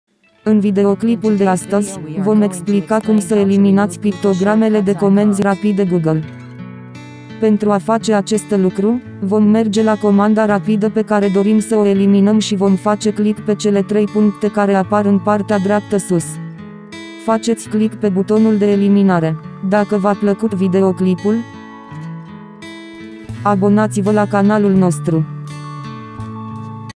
Cum să ștergeți comenzi rapide Google
Mini tutorial care explică într-un mod simplu cum să eliminați comenzile rapide sau miniaturile care apar în partea de jos a ecranului de pornire Google Chrome.